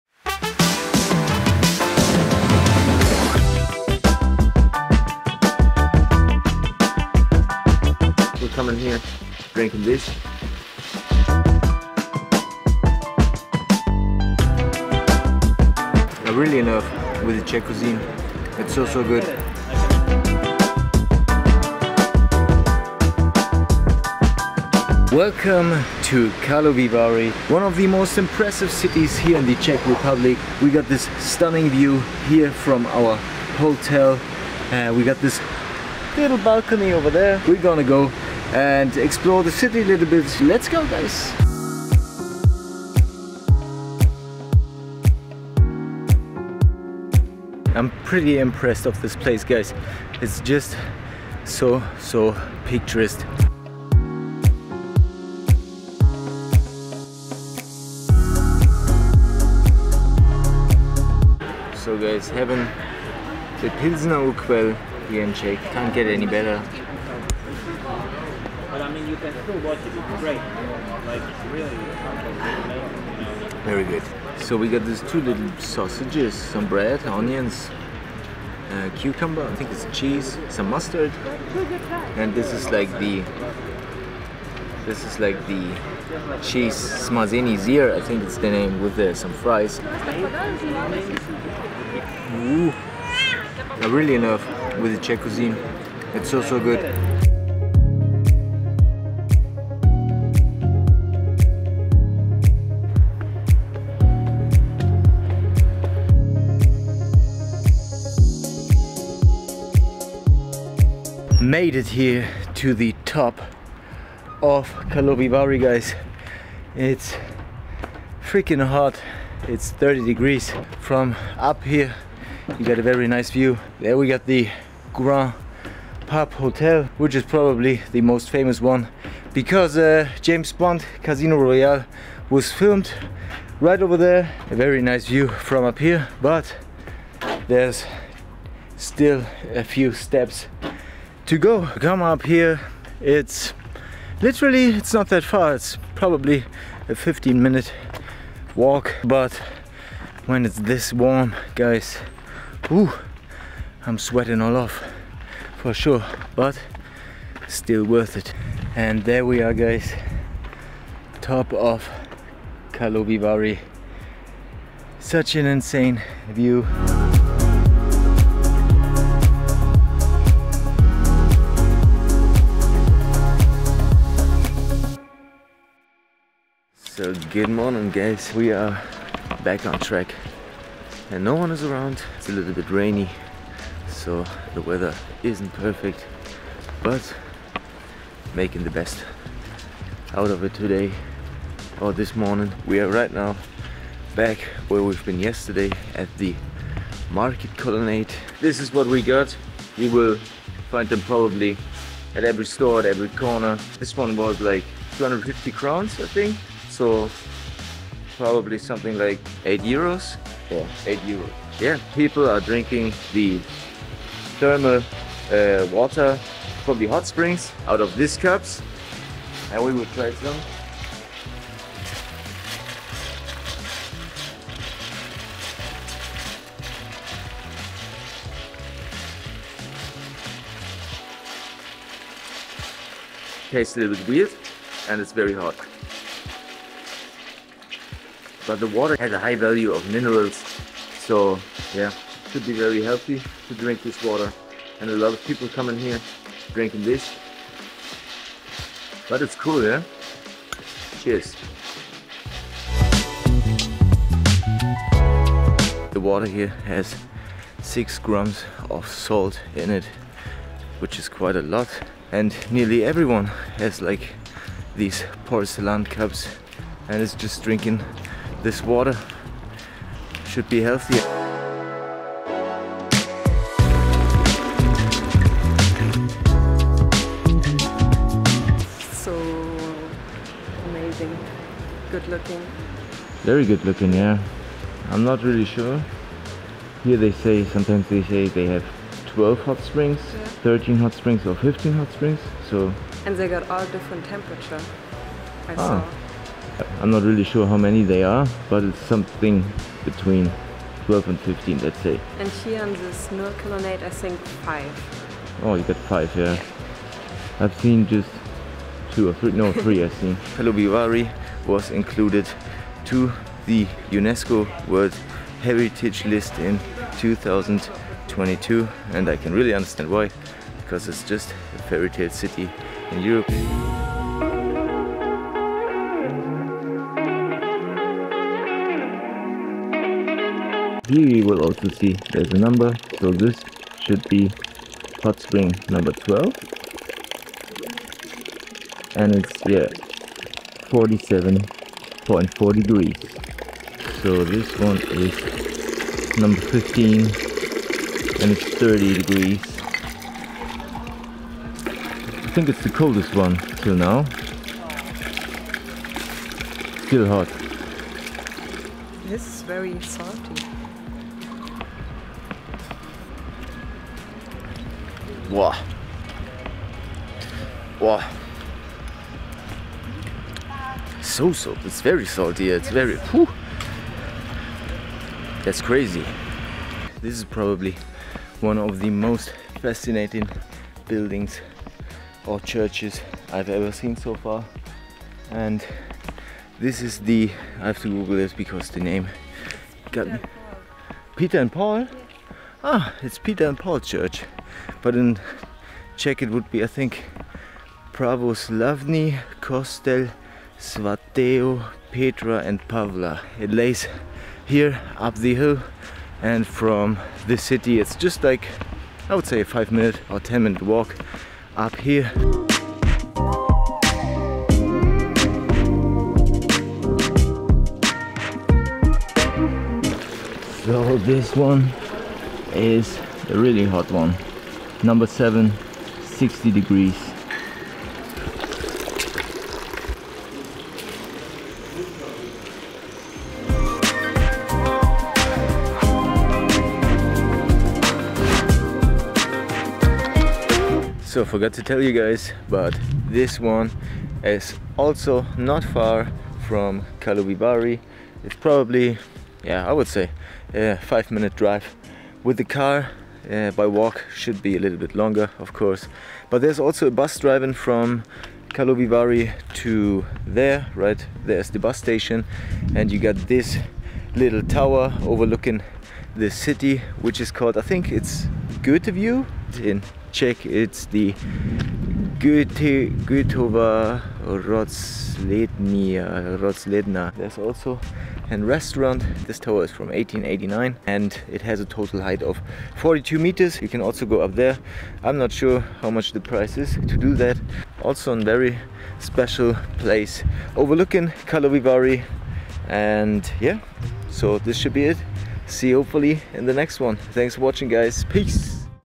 Karlovy Vary, Czech Republic I Honest Guide
A honest Guide to the fairytale city of Karlovy Vary in the Czech Republic.
Karlovy Vary is a health resort in the west of the Czech Republic with around 49,000 inhabitants. It is one of the most impressive and gorgeous cities in all over Czech Republic. In this video I want to give you some insights to this awesome place and show you what you can do in less then 24 hours.

My Camera Gear:

⚫️ Lens 24-70 mm
⚫️ Lens 28-70 mm
⚫️ Camera Lens Filter
⚫️ GoPro Half Dome
⚫️ Hard Disk